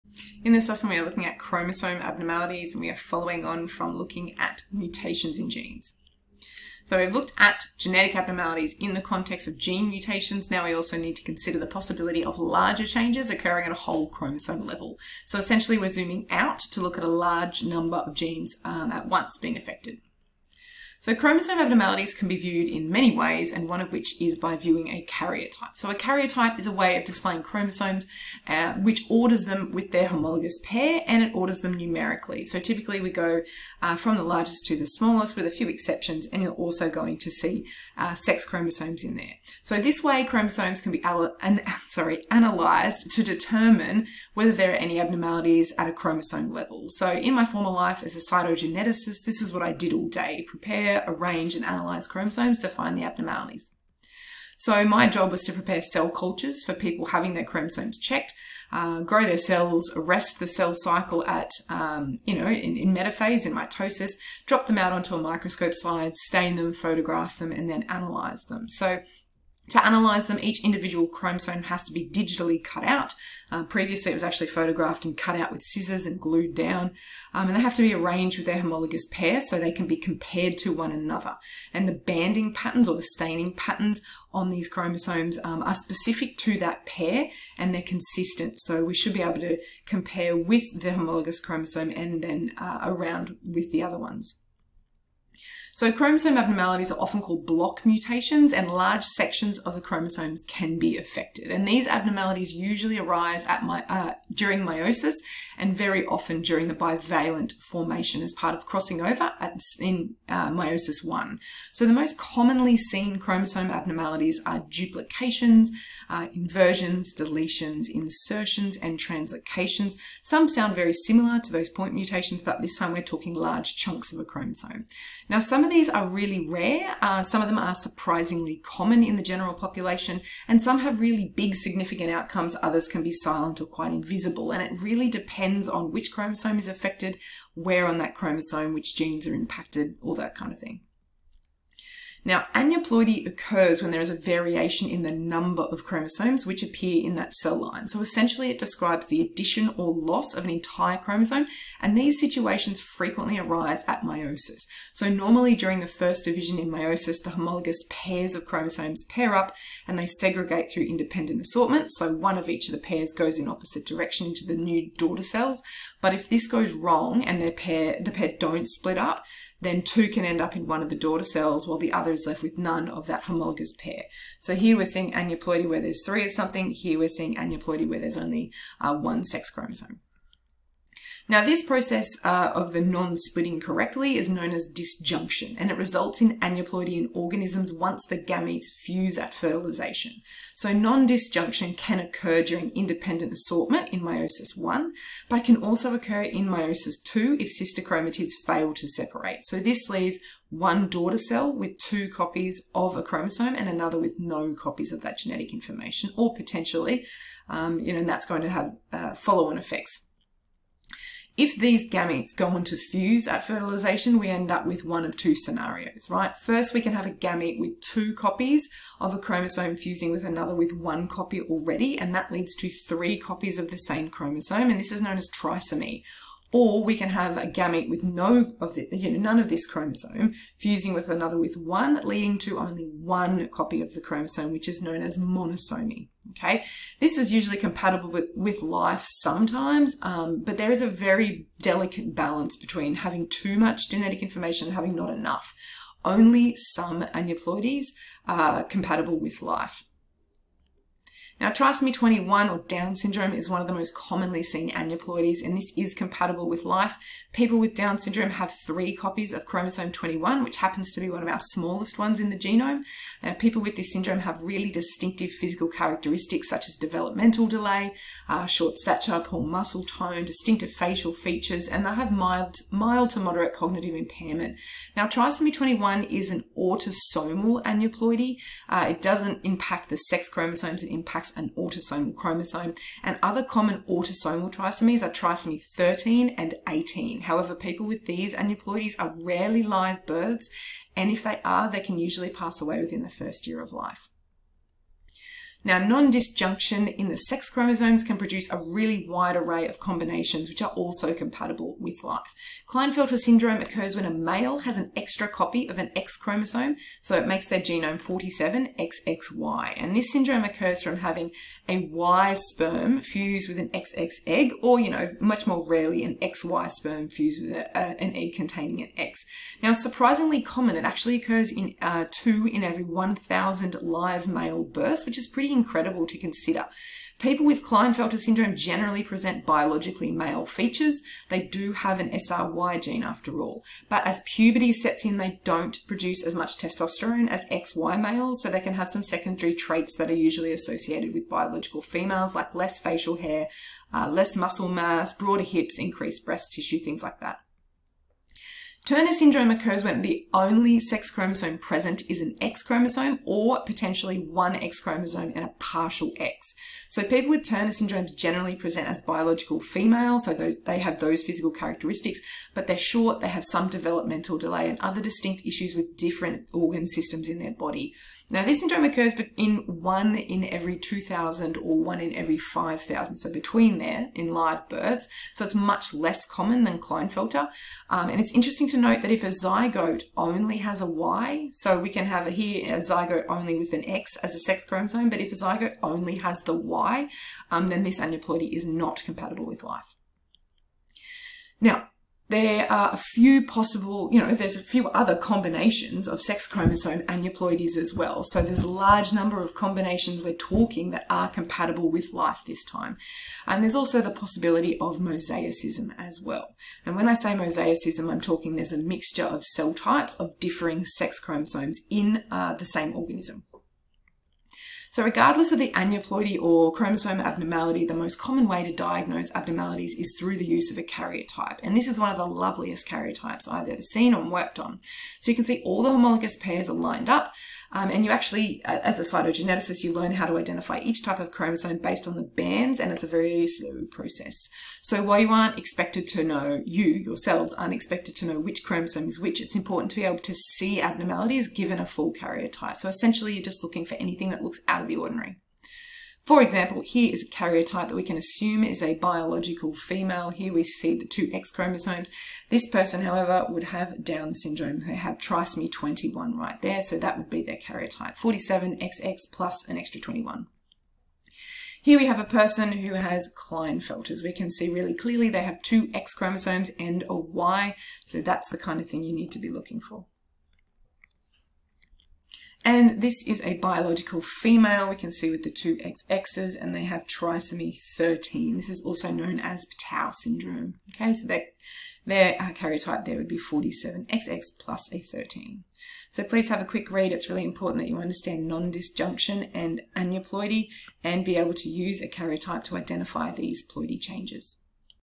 Chromosome abnormalities
QCAA Biology 2019. Unit 4, Topic 1

- identify how mutations in genes and chromosomes can result from errors in: cell division (non-disjunction)
- explain how non-disjunction leads to aneuploidy
- use a human karyotype to identify ploidy changes and predict a genetic disorder from given data